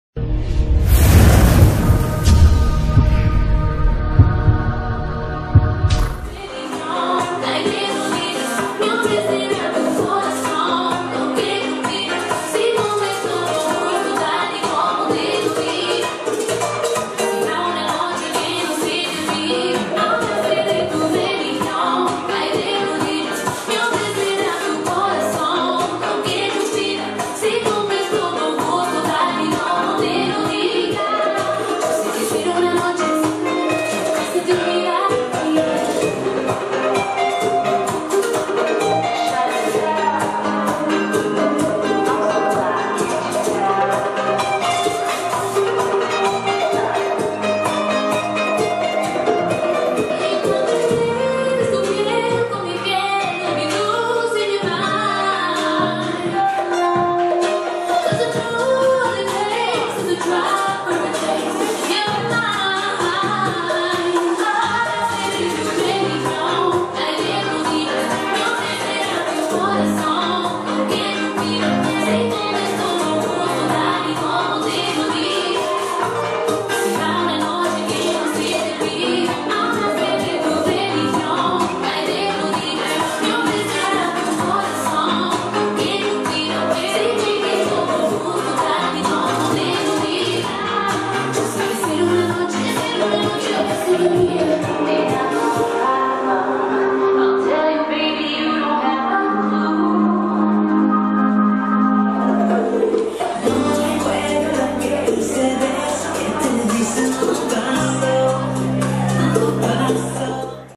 Alon Goshen & Eden Levy dance Bachata
Alon Goshen & Eden Levy dance Bachata in Bachata Nation, Israel.